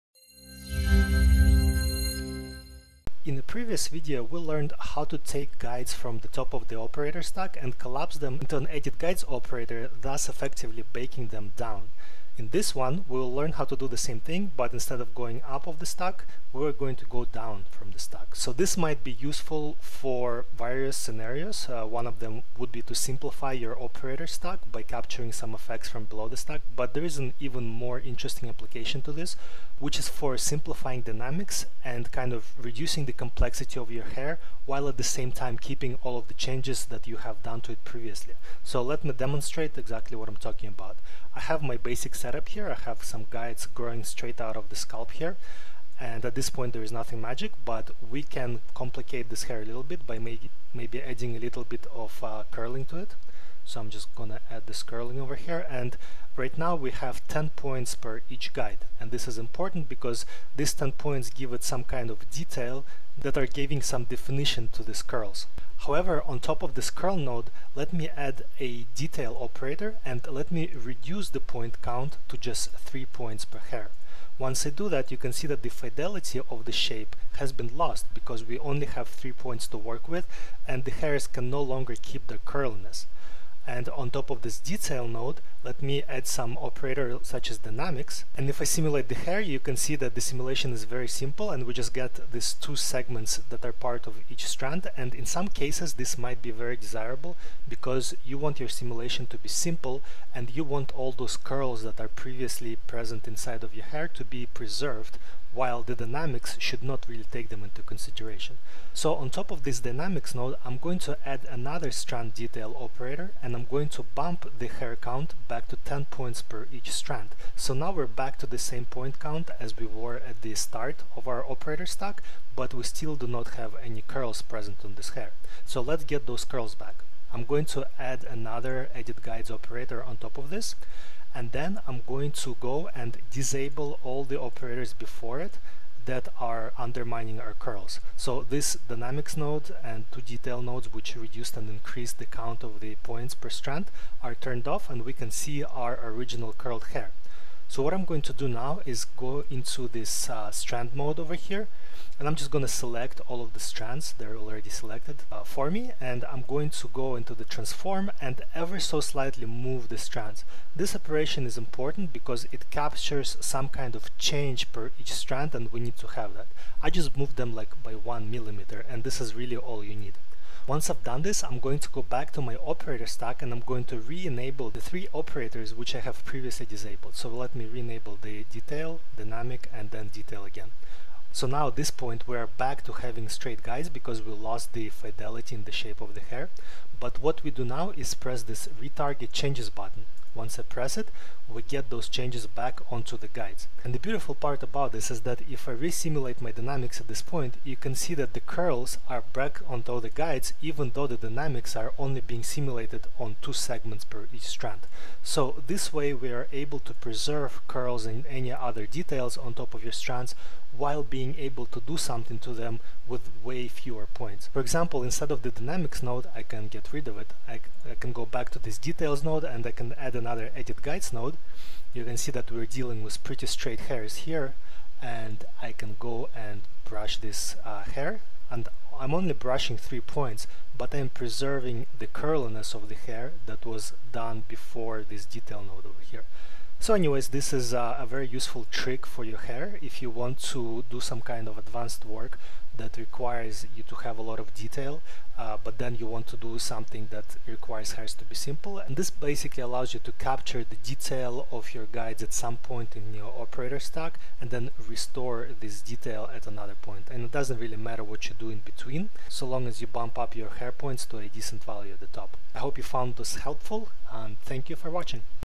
Ornatrix for Maya: Adopt changes from stack bottom
In this tutorial we go over a great grooming workflow technique which captures details from some position in operator stack and restores it at some other position later on. Use this useful trick to improve the quality of your hair grooms and simulations.

Ornatrix is a complete hair, fur, and feather solution for Autodesk Maya.